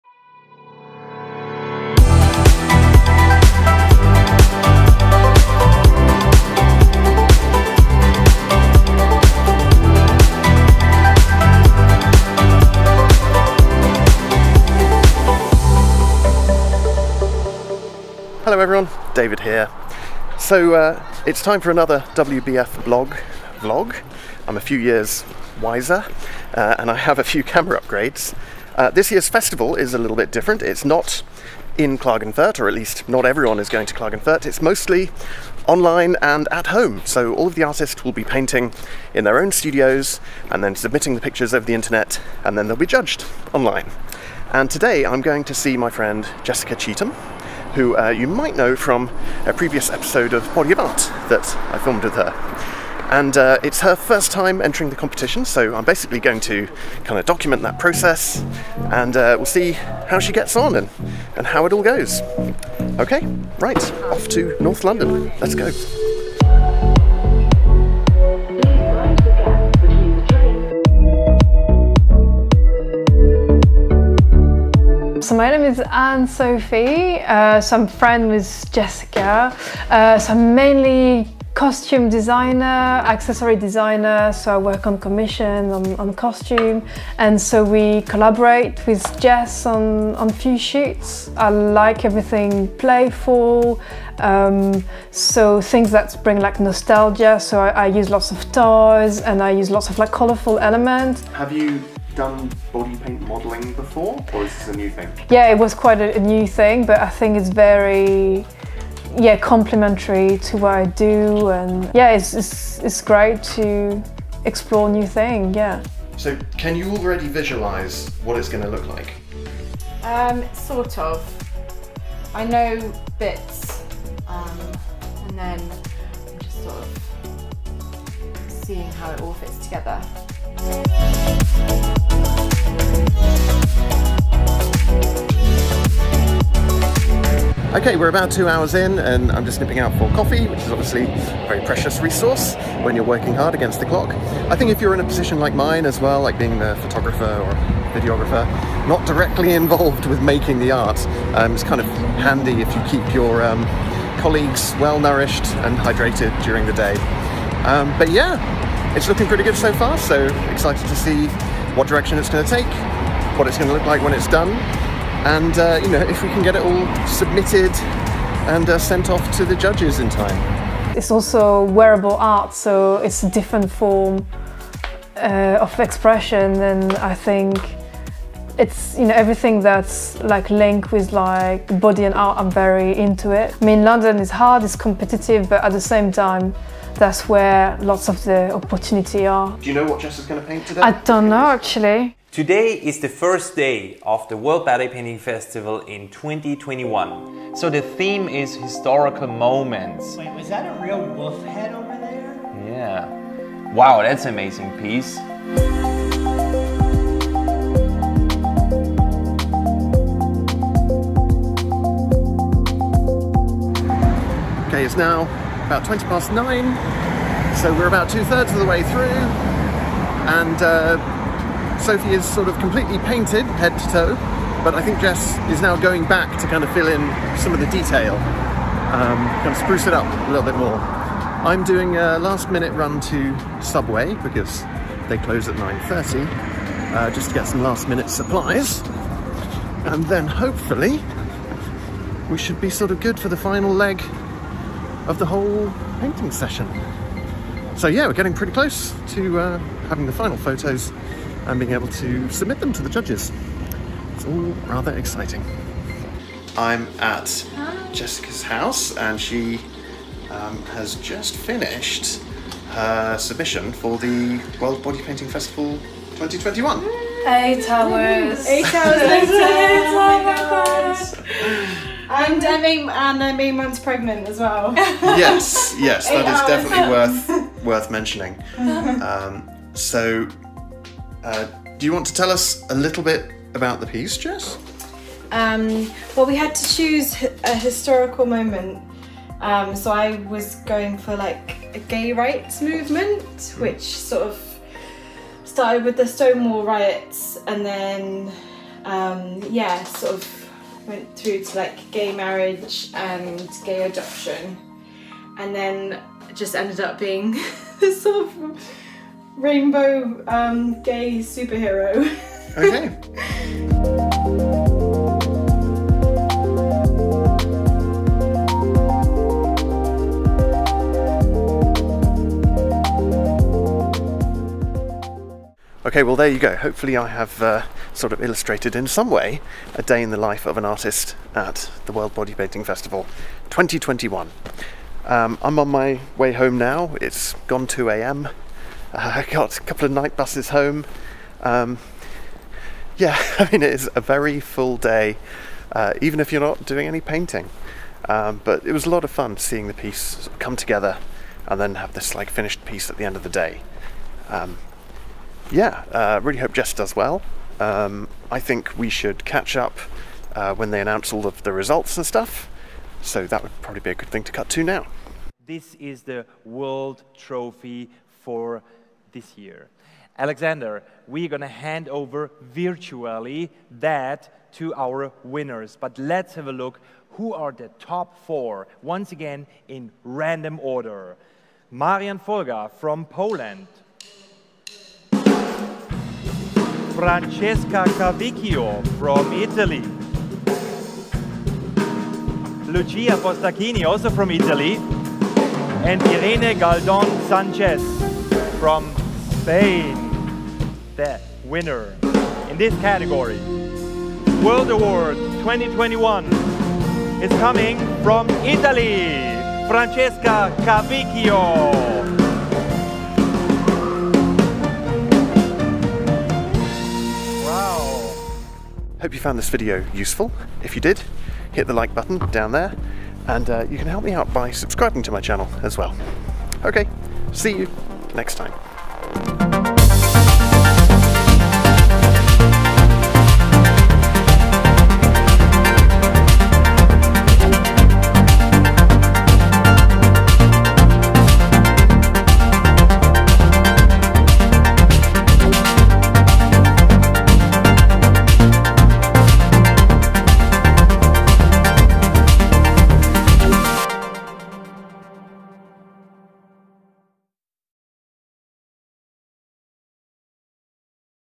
WBF 2021 Blog - Jessica Cheetham’s Journey
This year's World Bodypainting Festival is a bit different - it's at home and online! So I decided to reach out to my friendly neighbourhood bodypainter, and I chart her progress as she enters the competition for the first time (while 8 months pregnant, no less).

Timecodes:
0:00 Intro
1:15 Interview with Sophie
2:17 2 Hours In
2:50 Powering Through
3:11 Alex's Recap
3:25 Work Continues
3:34 5 Hours In
4:16 Finished!
5:25 Heading Home
6:05 Awards Ceremony
6:58 Outro

Equipment Used:
Sony A7c with 28-200mm Tamron Lens
Sony Zeiss 55mm Lens